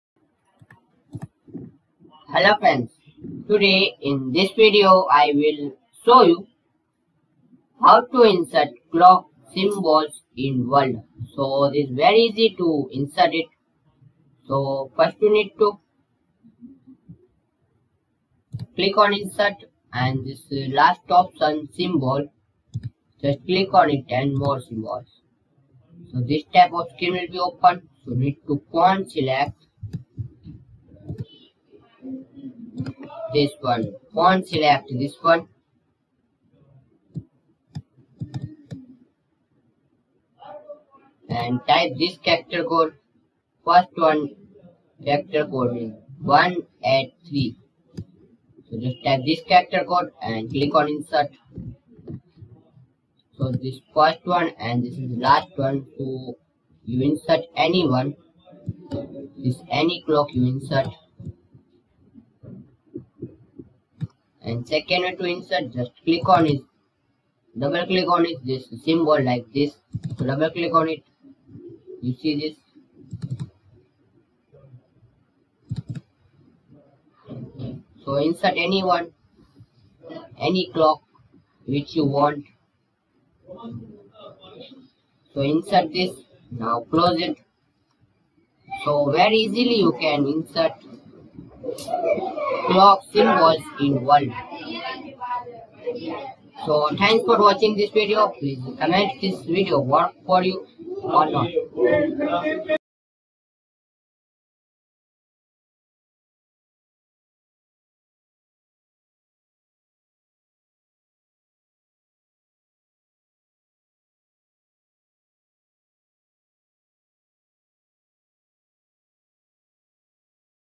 How to Insert Clock Symbols in Word
Learn how to easily insert clock symbols in Word with this step-by-step tutorial. Whether you're writing reports or creating schedules, adding clock symbols can enhance your document and make it more visually appealing. Follow along to learn the simple process of inserting clock symbols in Microsoft Word!